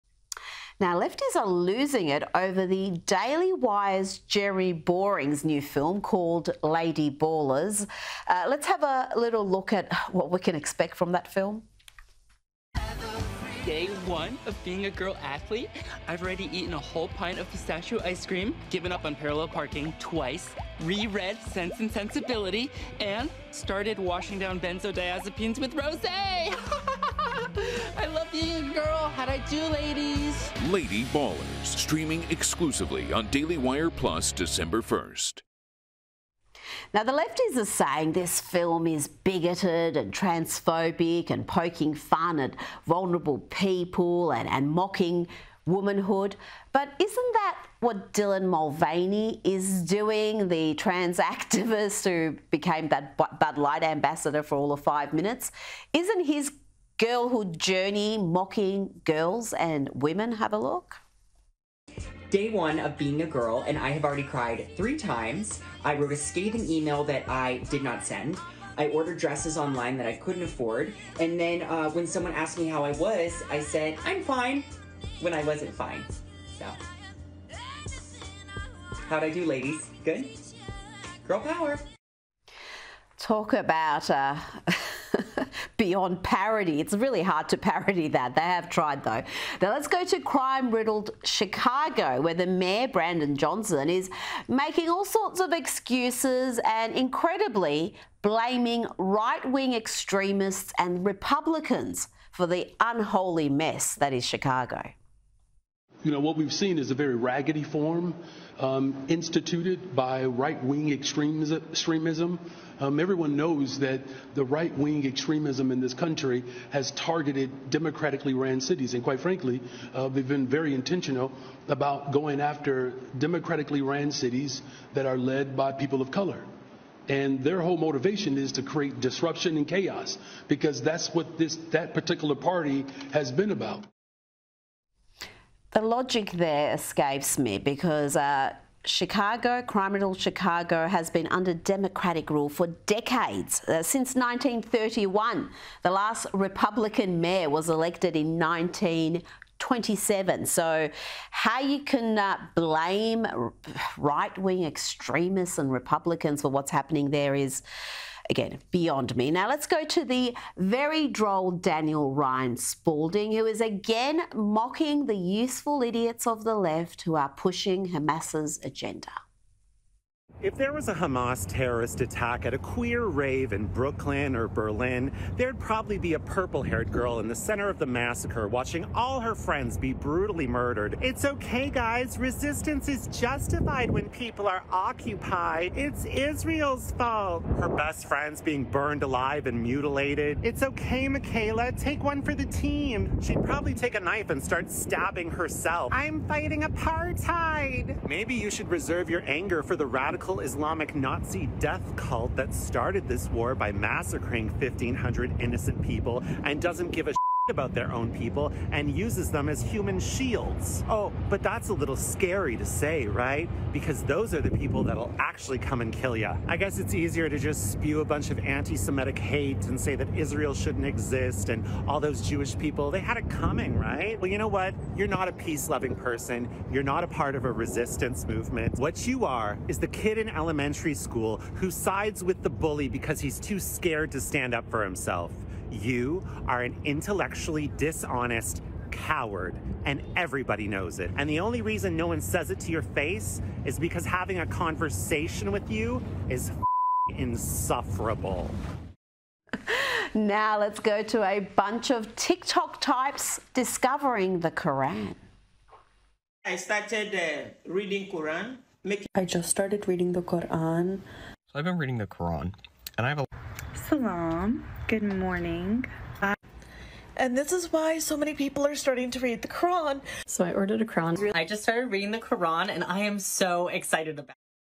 Lefties losing it: Comedian mocks the 'useful idiots of the Left' pushing Hamas' agenda
Sky News host Rita Panahi has reacted to comedian Daniel Ryan Spalding mocking the “useful idiots of the Left” who are pushing Hamas’ agenda.

“It’s ok guys, resistance is justified when people are occupied,” Ms Spalding mocked in the video posted to social media.

“It’s Israel’s fault.

“Maybe you should reserve your anger for the radical Islamic Nazi death cult that started this war.

“Oh, but that’s a little scary to say, right? Because those are the people that’ll actually come and kill you.”